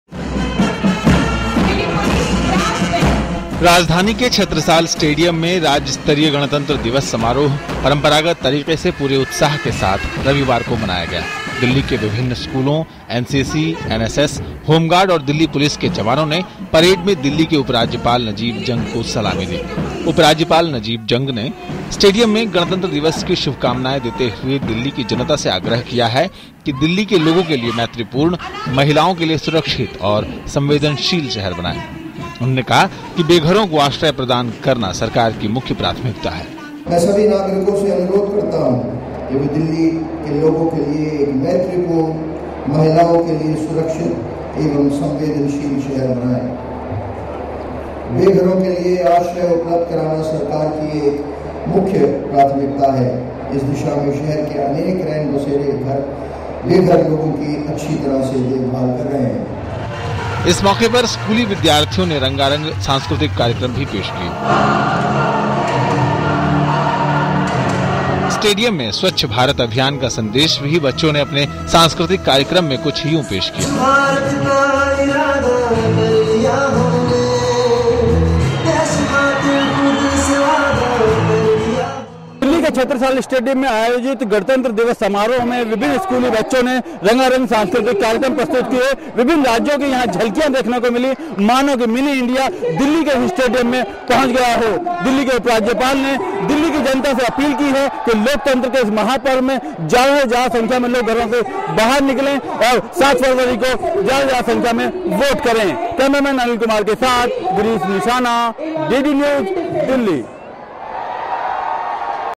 Delhi State celebrates Republic Day
आज दिल्ली के छत्रसाल स्टेडियम में गणतंत्र दिवस समारोह मनाया गया। इस मौके दिल्ली के उपराज्यपाल नजीब जंग ने दिल्ली की जनता से मैत्रीपूर्ण और महिलाओं के लिए सुरक्षित दिल्ली बनाने के लिए अनुरोध किया है। पेश है हमारे संवाददाता गिरीश निशाना की एक रिपोर्ट-